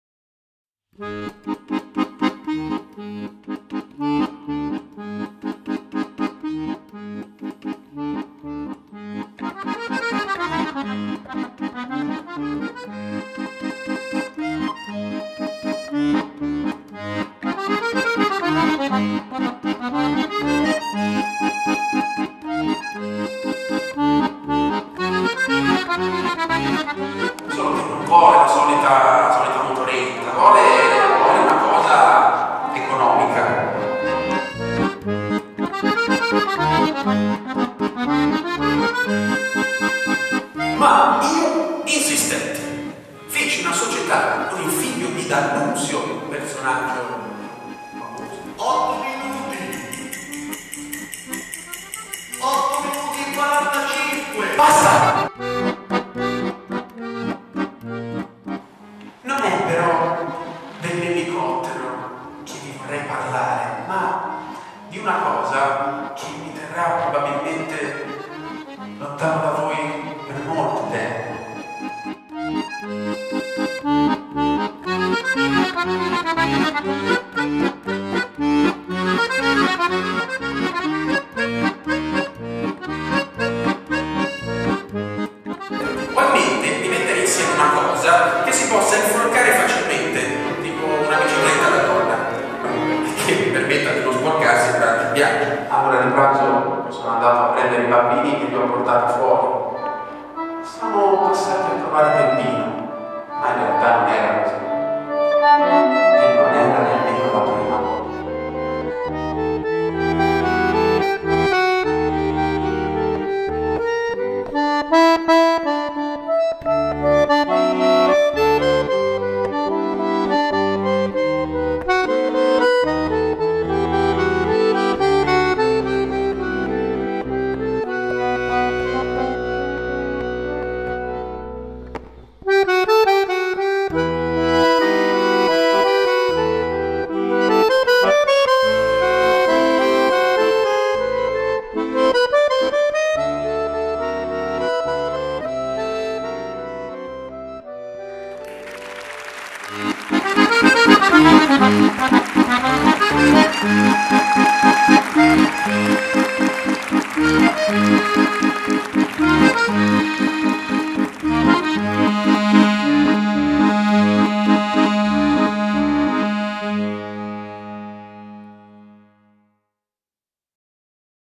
"I SOGNI NON DEVONO MORIRE MAI" di M.Cristiani (trailer)
..una breve piece teatrale che vuole raccontarci la vita di un personaggio poco noto,ma di eccezionali  doti. Corradino d'Ascanio fu l'inventore dello scooter VESPA oltre che del primo prototipo di ELICOTTERO!!!   testo di M.Cristiani       interpretata da Carlo Bernardini e musicata da Fabio Ceccarelli….